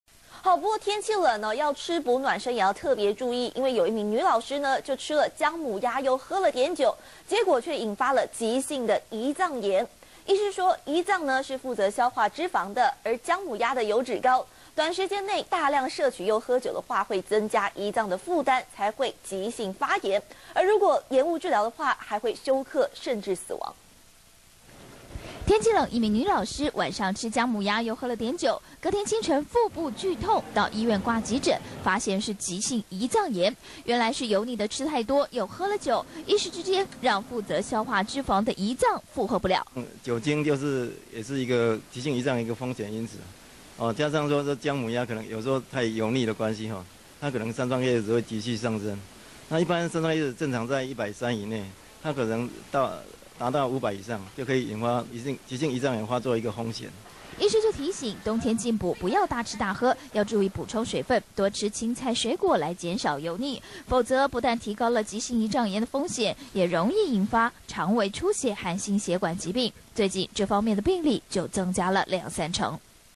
吃薑母鴨又飲酒 女進補急性胰臟炎
中視新聞》天氣冷，不少民眾愛吃補暖身，一名女老師吃了薑母鴨又喝了酒，引發急性胰臟炎。醫師說，胰臟負責消化脂肪，而薑母鴨油脂高，短時間內大量攝取又喝酒增加胰臟的負擔，才會急性發炎，如果延誤治療，還會休克甚至死亡。